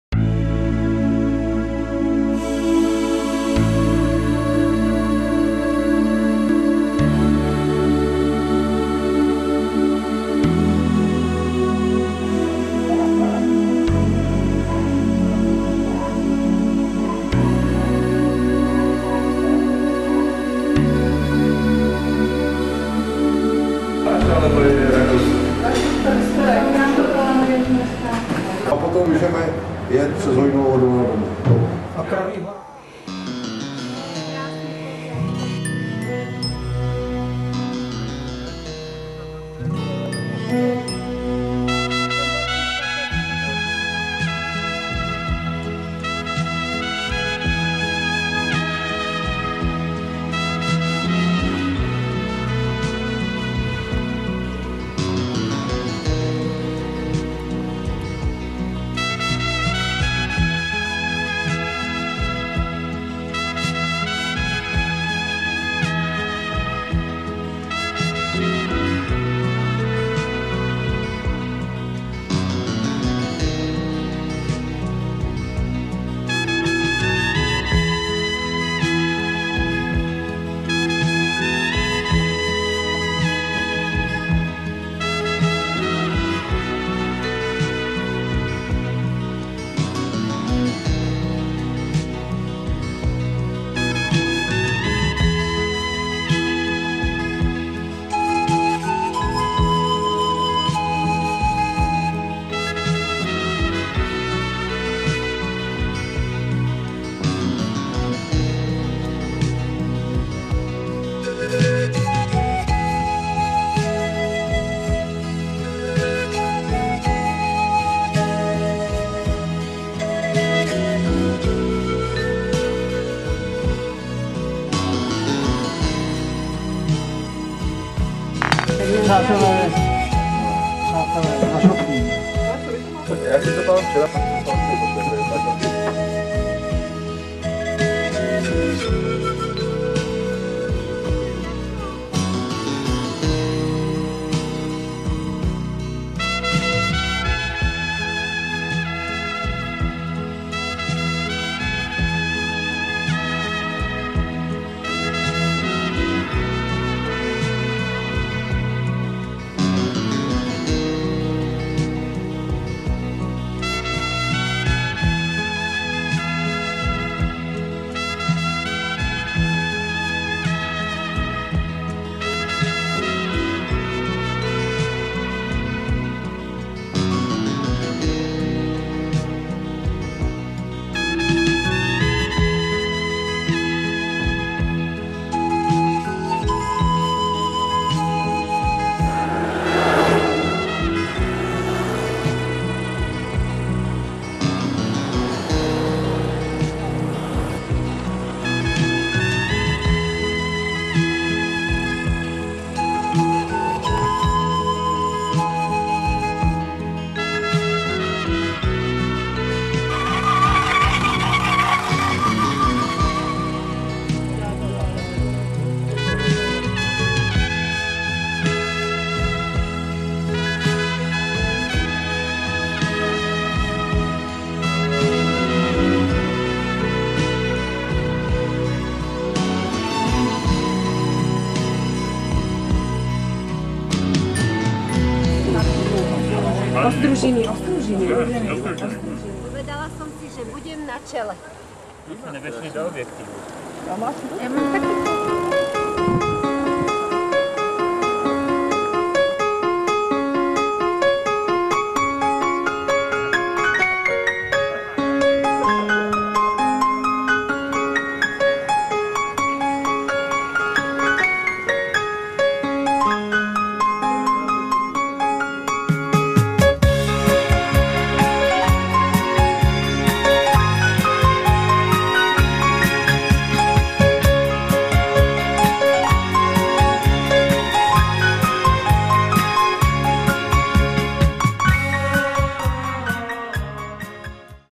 Přes Rakousko na Žofín - v Novohr. horách 2.9.2011
Výlet na kolech do  Rakouska na vyhlídku Mandelsteine a přes Žofín a Dobrou vodu zpět do Nových Hradů.